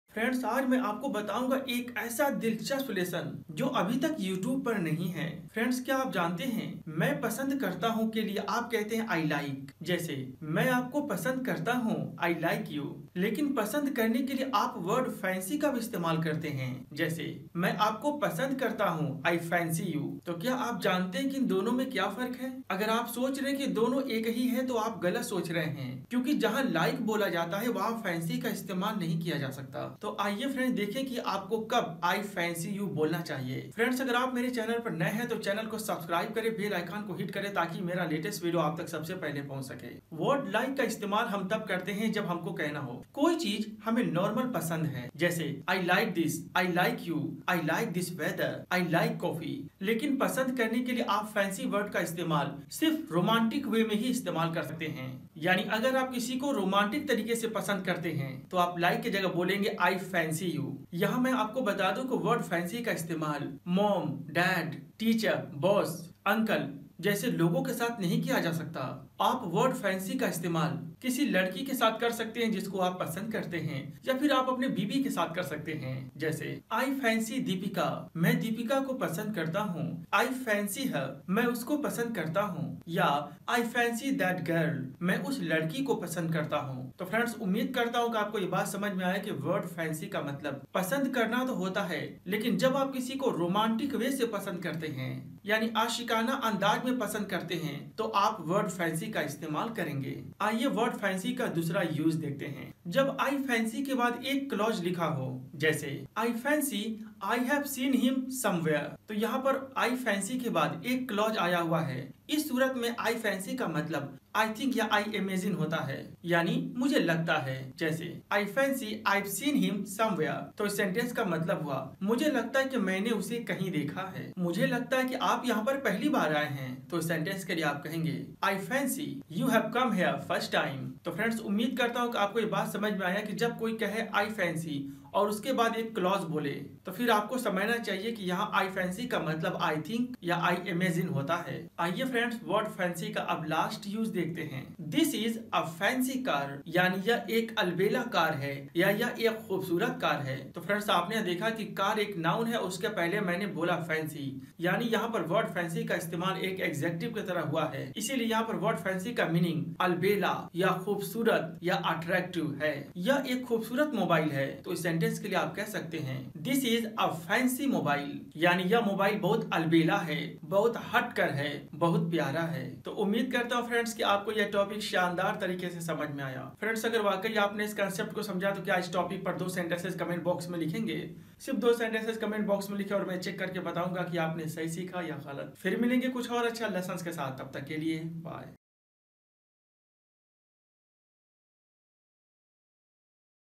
Use of I fancy | Fancy uses in English | इंग्लिश सीखे वीडियो | English by spoken english sir
I fancy /I like (पसंद करना)

🗒 Note :दोस्तो आज हमलोग सीखेंगे एक बहुत ही शानदार lesson जिसे इससे पहले कभी नहीं पढे होंगे। दोस्तो "I fancy /I like" दोनों  का मतलब पसंद करना होता है लेकिन जहां "I like" का use करना है हम वहां "I fancy" का use नहीं कर सकते, दोस्तो "I fancy" का use हम आशियाना अंदाज में किसी girl , wife के लिए करते हैं इसका हम मम्मी, पापा, भाई, बहन, चाचा, चाची, के साथ हम "I fancy" का use नहीं करते हैं। "I like" normally किसी को अगर हम पसंद करते है बोल सकते है।

Ex. I fancy my wife.
       I fancy Deepika.

2. चलिए दोस्तो "I fancy" second use देखते है Friends 'I fancy' के बाद अगर कोई clause आए तो I fancy का मतलब "I think" होता है।

Ex. I fancy, you are new here.

3. दोस्तो अब हम सीखेंगे " fancy" का third use सीखे
की जब हमे कहना हो कि कोई समान बहुत प्यारा, अलबेला है तो हम "fancy" का Use करते है।

Ex. बृजेश एक  अलबेले लड़का है
Brijesh is a fancy boy.